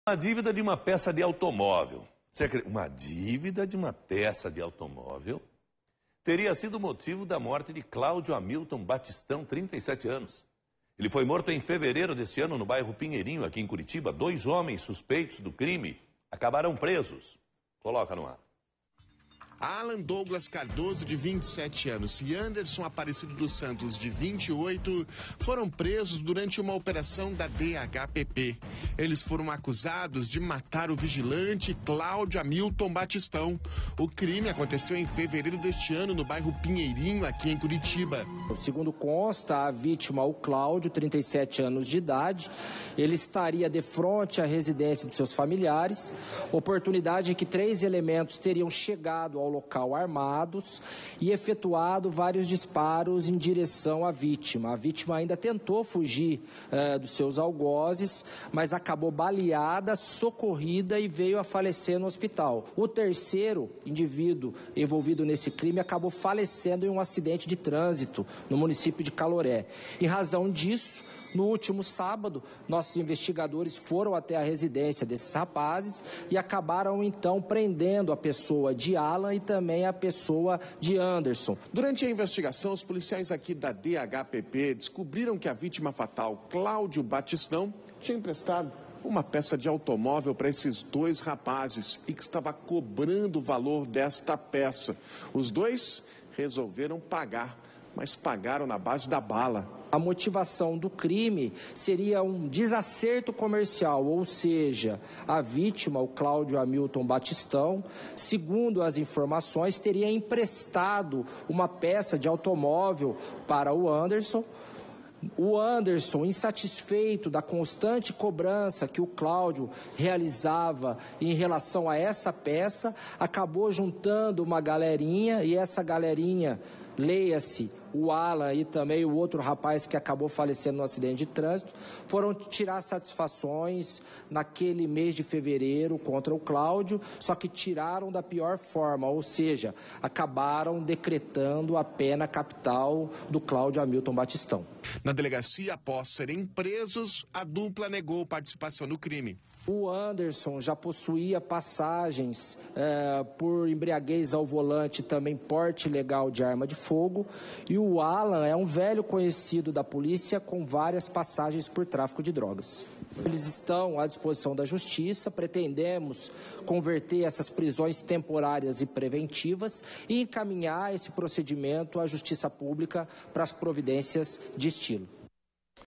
Dívida por peça de automóvel causa morte no Pinheirinho
Um homem de 37 anos foi vítima de dois suspeitos que teriam assassinado por motivo de cobrança de uma peça de automóvel no bairro Pinheirinho, em Curitiba.